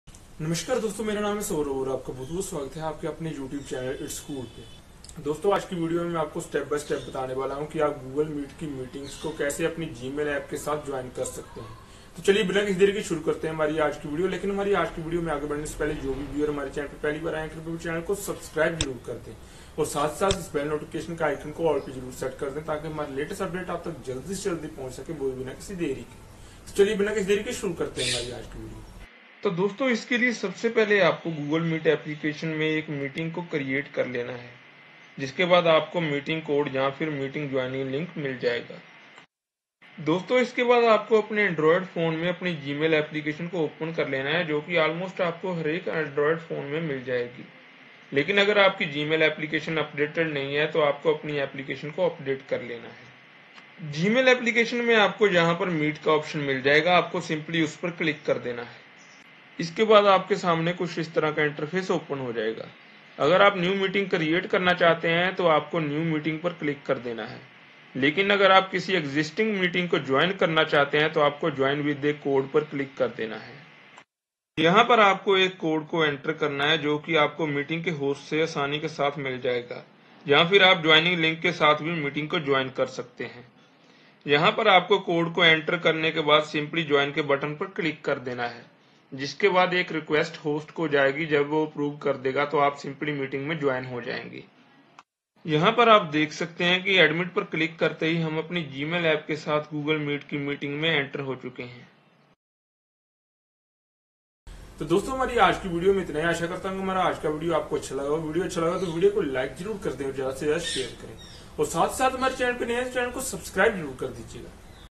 How to Join Meeting on Google Meet with Gmail Application | Use Google Meet With Gmail 2020
I this video I will show you how you can join a google meet meeting with gmail application in step by step in 2020